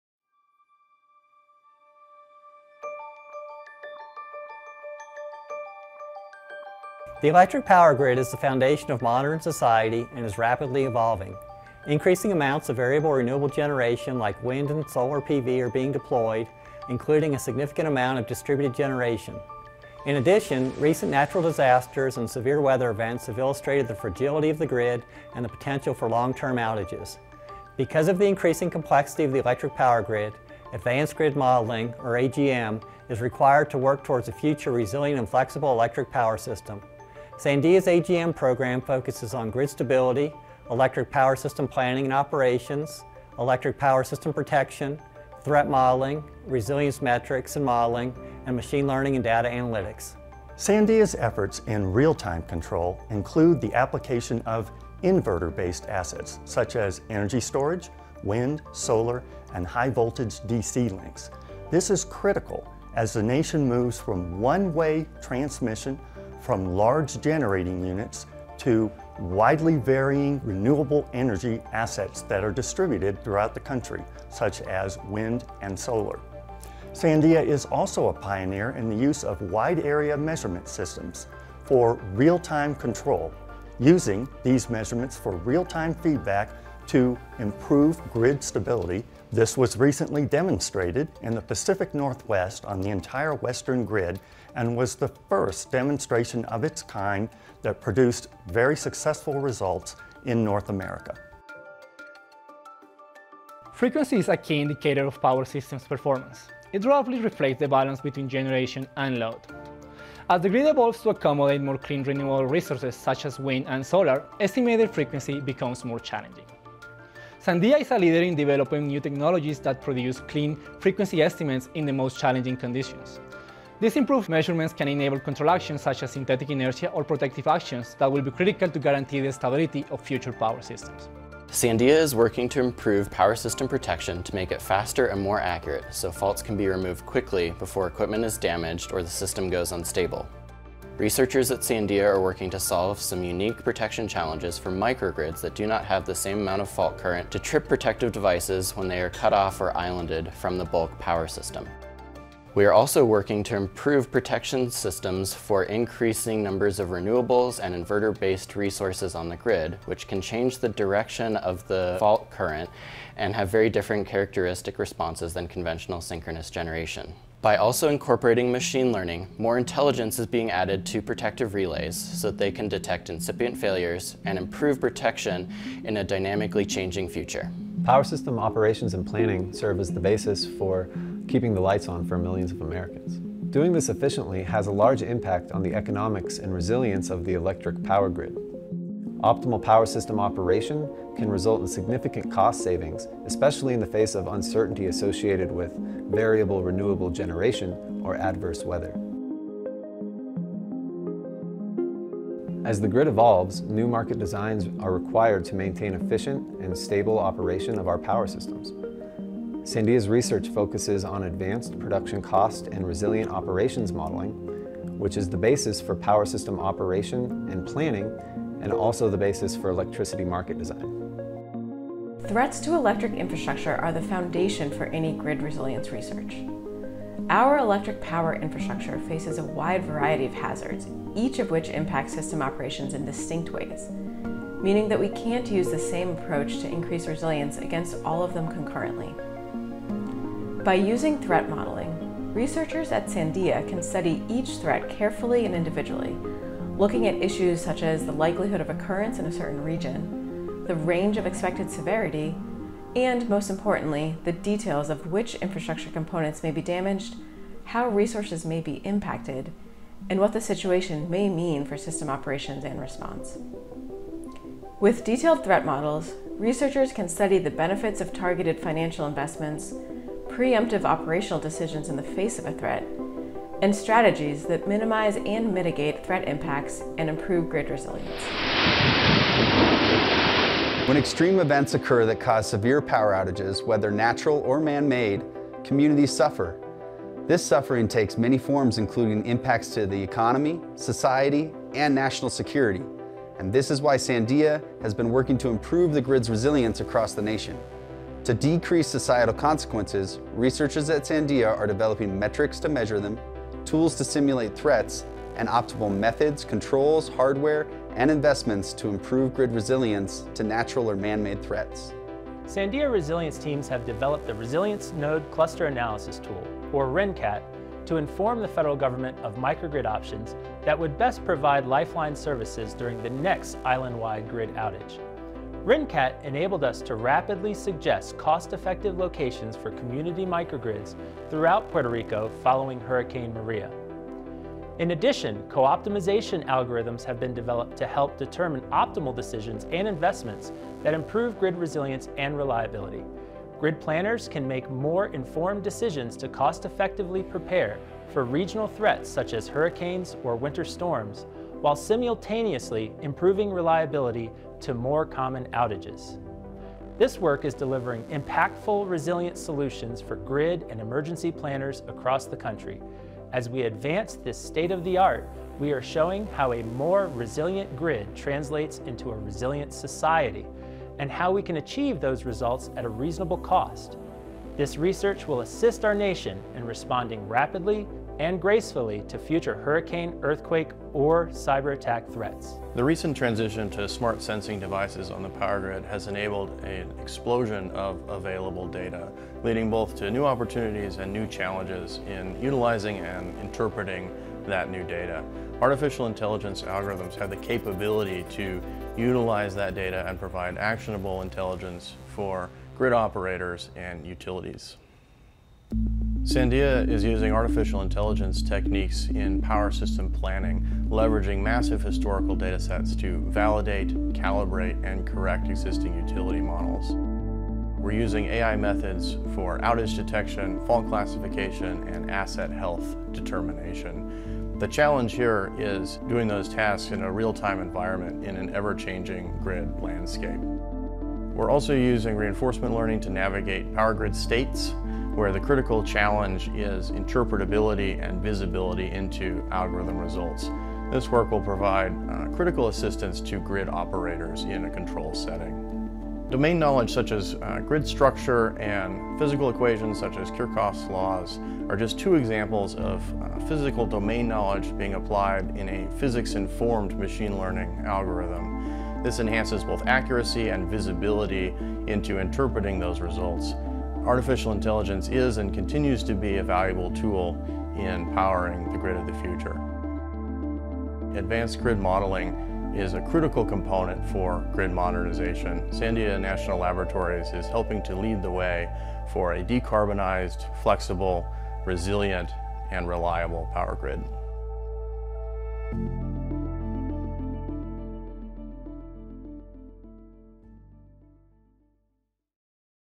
Advanced Grid Modeling at Sandia
Because of the increasing complexity of the electric power grid, advanced grid modeling is required to work towards a future resilient and flexible electric power system. Sandia’s Advanced Grid Modeling program focuses on grid stability, electric power system planning and operations, electric power system protection, threat modeling, resilience metrics and modeling, machine learning, and data analytics.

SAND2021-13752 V